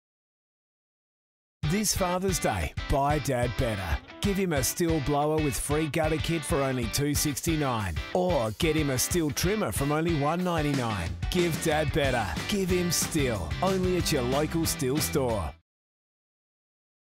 Dad deserves something better this Father's Day!
What's better? An awesome garden, created by your dad - with the help of a super powerful STIHL. There's special offers on a range of STIHL grass trimmers, blowers, hedge trimmers and heaps more.